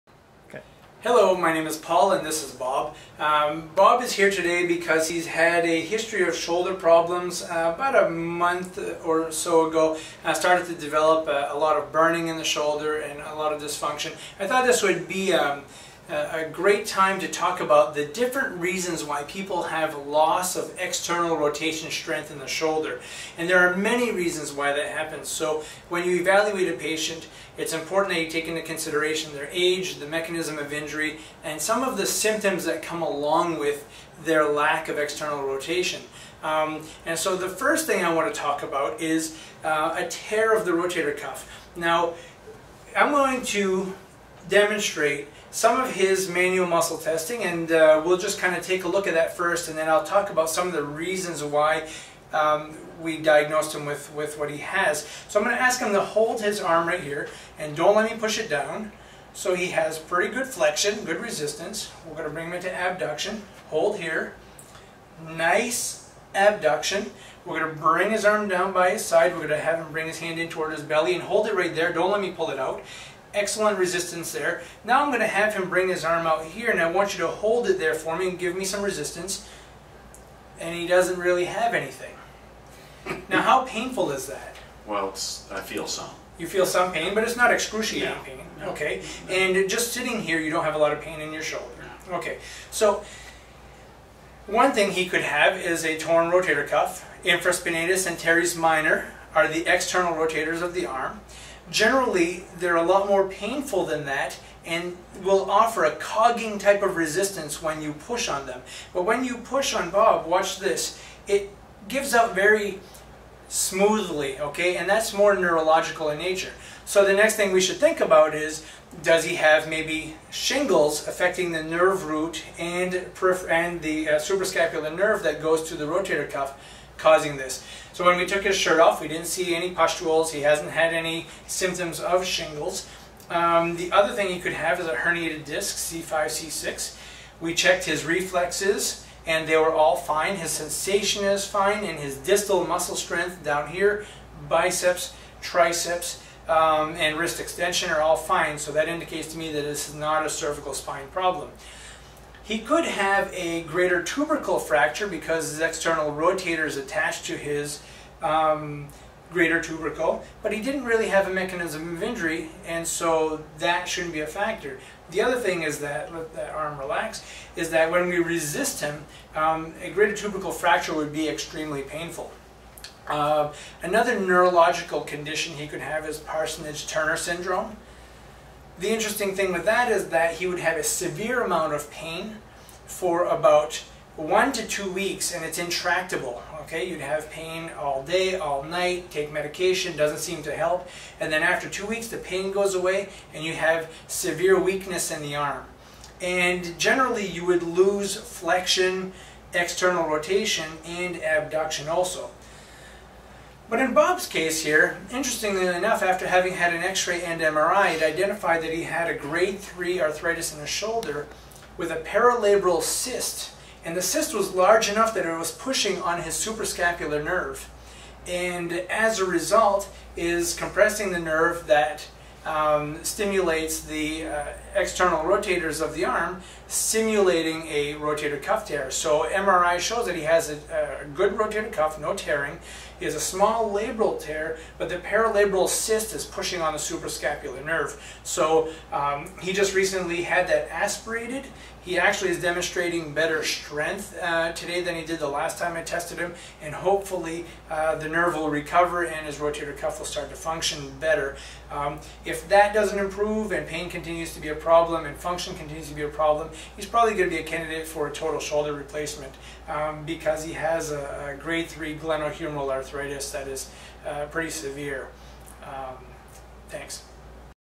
Suprascapular Nerve Injury Caused by Supra Labral Cyst
Here is a brief explanation of 6 different reasons why a person may have shoulder external rotation weakness and a patient with a suprascapular nerve injury. This patient had a supra labral cyst secondary to glenohumeral arthritis pressing on the suprascapular nerve.

Follow our Podcast show on Apple Podcasts, Spotify and most all other podcasting platforms. Just search: Ortho Eval Pal